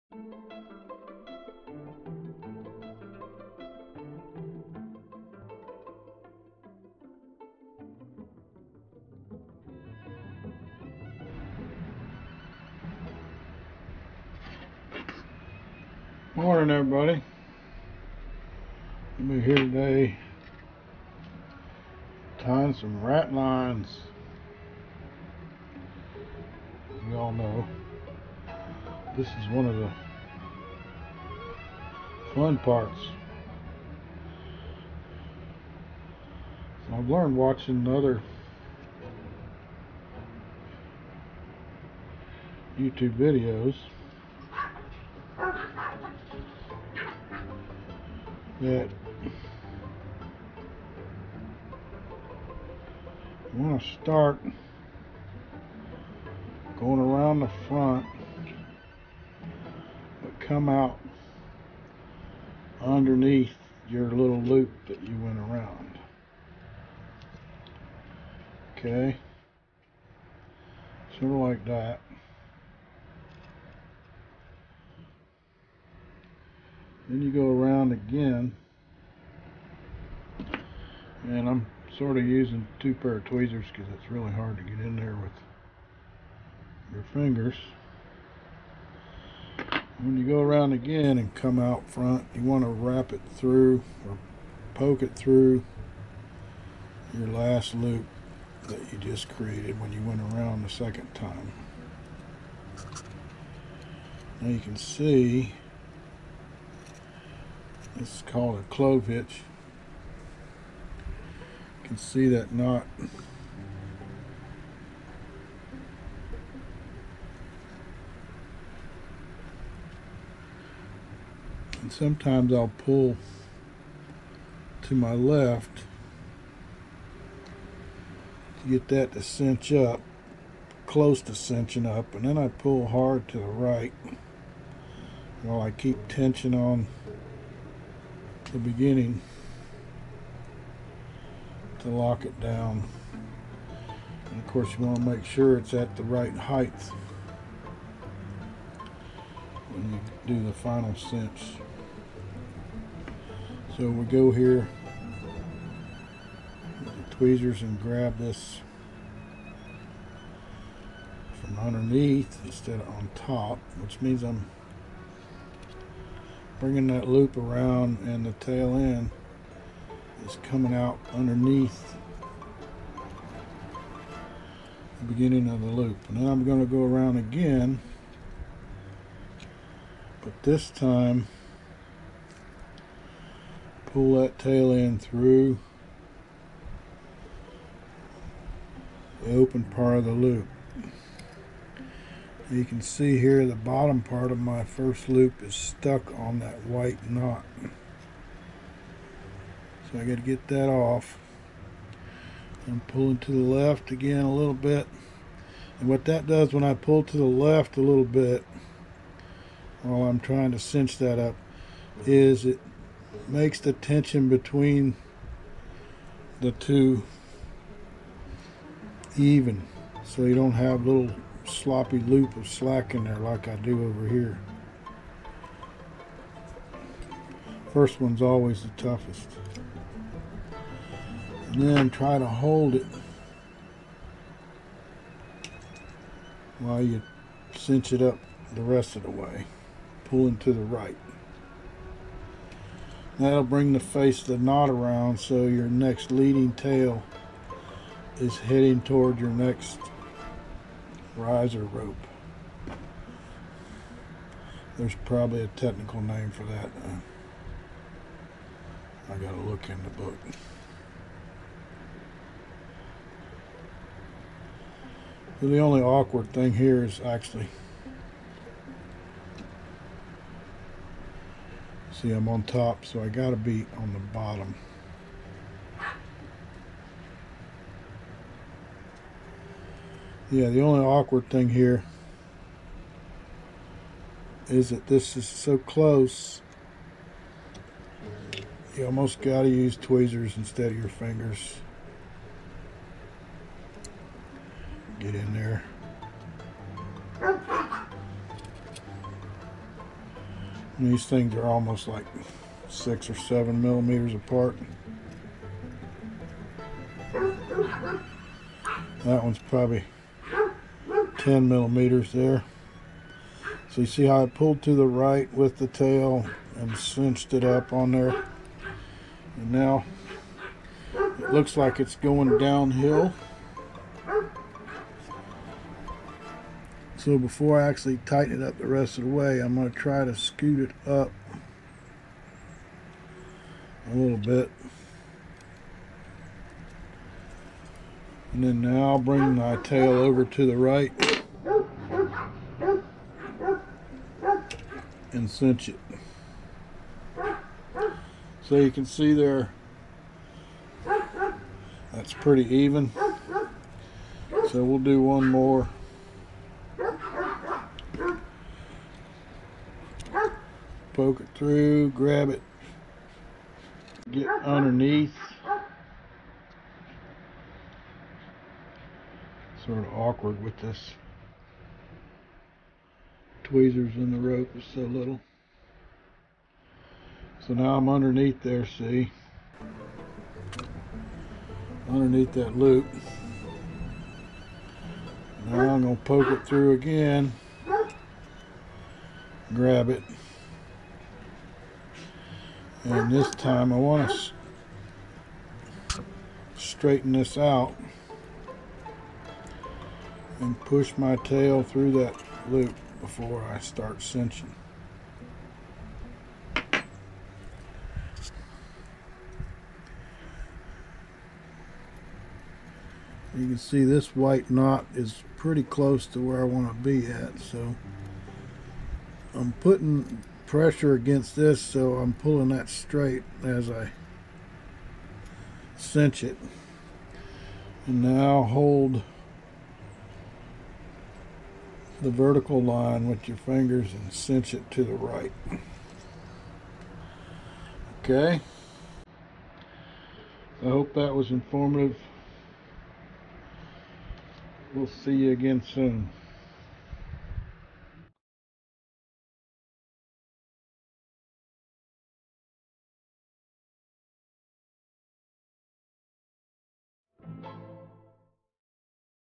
Wooden Model Ship Vlog # 20
Varmint lines. Approximately 198 rat lines total, and I'm not even half way yet. Wooden model ship SAN JUAN NEPOMUCENO kit manufactured by ARTESANIA LATINA.REF.:# 22860 this was a SPANISH WAR SHIP FROM THE YEAR 1765  she weighed 2700 tons